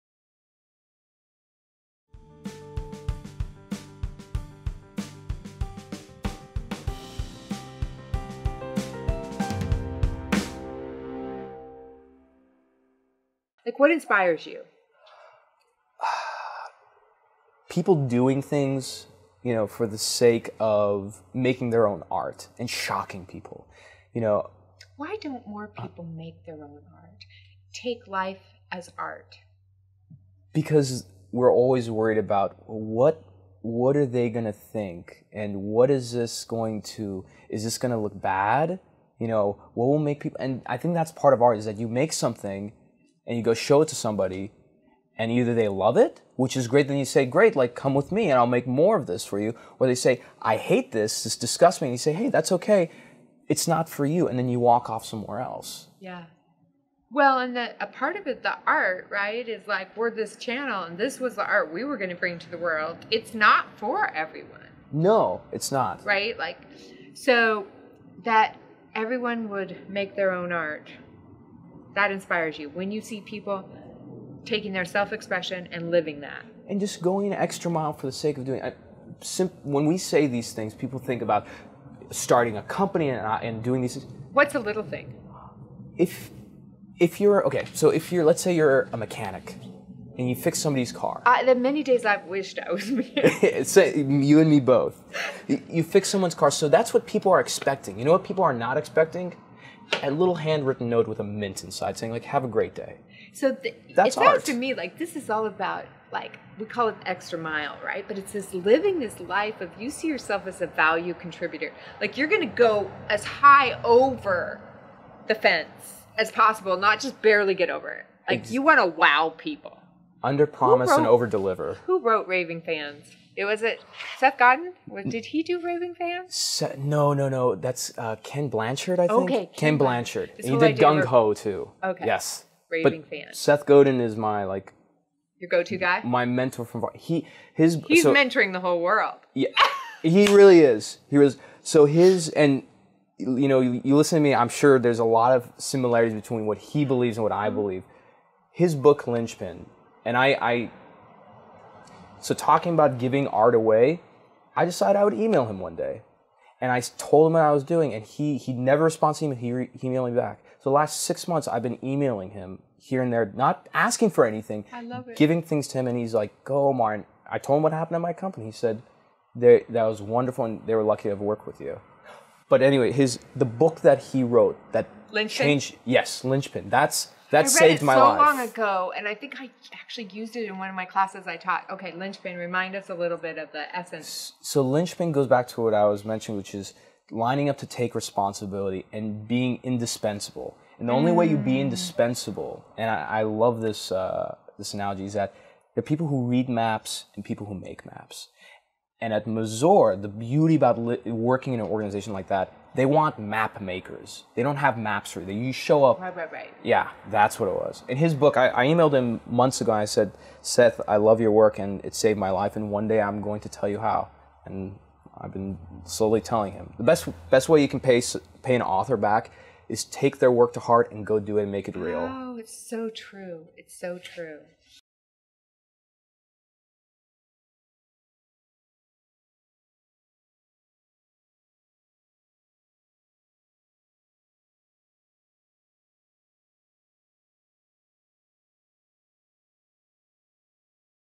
Life Is Art
Omar Khateeb and his business partner Adam launched P.S. Mister, an inventions company in men's fashion - just 6 months ago. In this video Omar shares what inspires him- people expressing themselves, living as if their life was, well their art. Be sure to hear the book that changed things for Omar and his mantra's in this excerpt from his interview with Pam Hoelzle during his Dear Rockstar podcast.